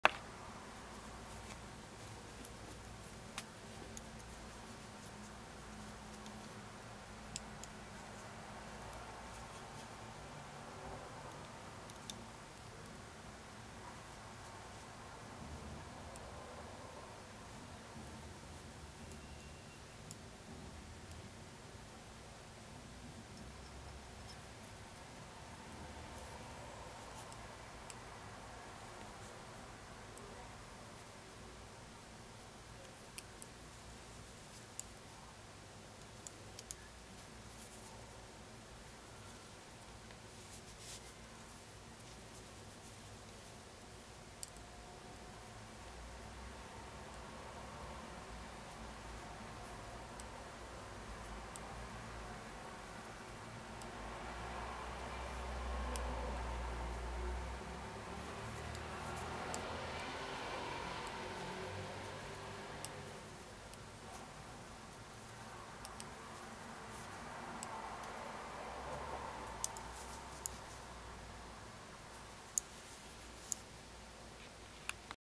걸러뜨기를 이용한 배색무늬 1
Debbie Bliss knitting magagine fall/winter 2010에 실린 'Hot Water Bottle Cover'에 사용된 무늬입니다. 자세한 설명과 사진은 블로그에서 확인하세요.